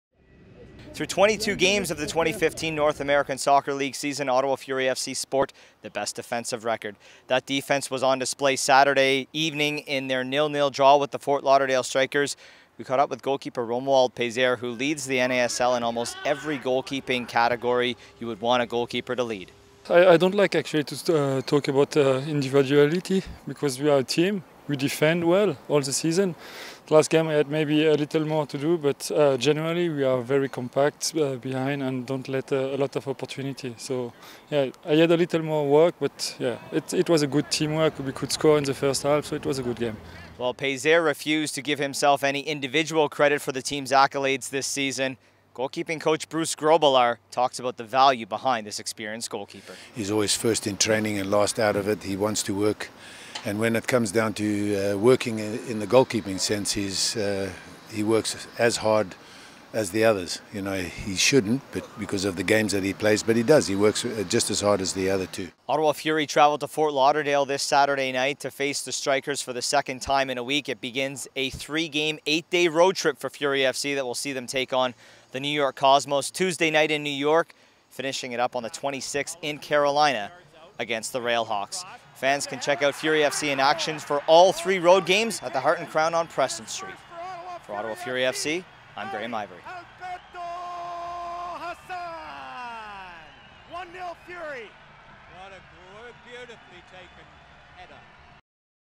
Peiser's record-setting season leading Ottawa's run towards the post-season
Fury FC goalkeeper Romuald Peiser earned NASL Team of the Week honours by securing his  his league-leading 11th shutout of the season in last Saturday's 0-0 draw with Fort Lauderdale

Graeme Ivory caught up with Peiser and goalkeeper coach Bruce Grobbelaar to discuss his strong 2015 campaign.